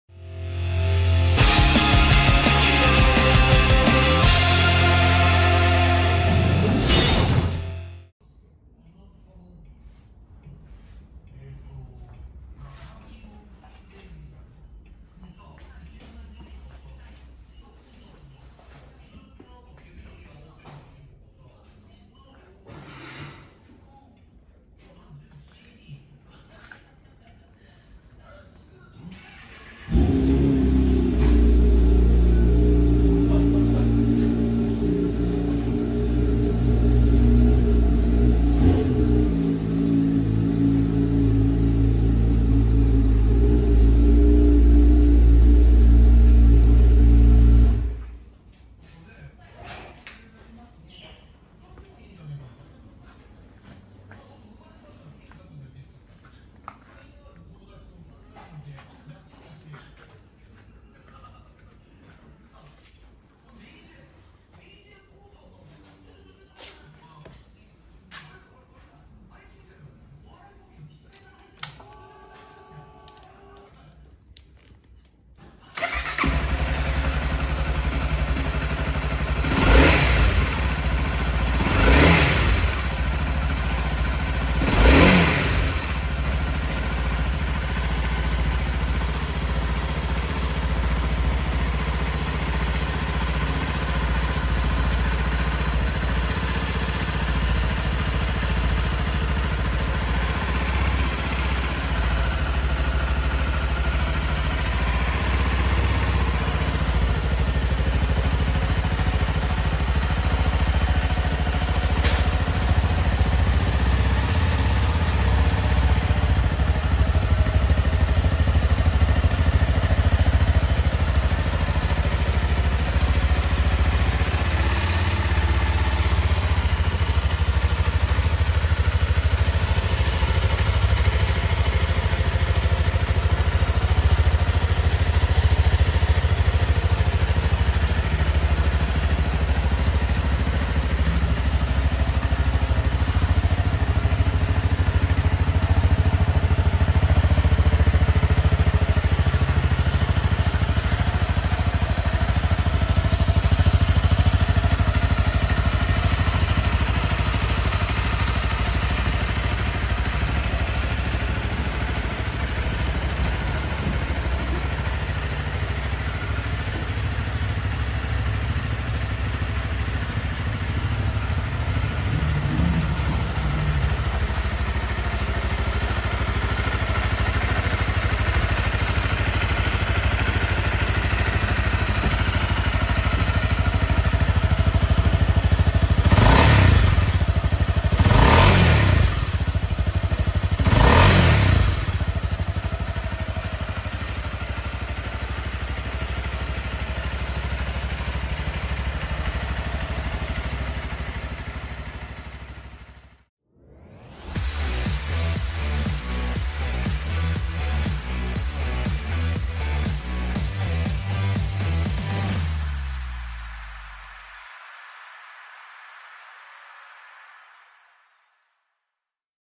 ホンダ HONDA ＣＲＦ２５０Ｌ エンジン音 2015年モデル！アルミリアキャリア付き
各部詳細画像はこちら！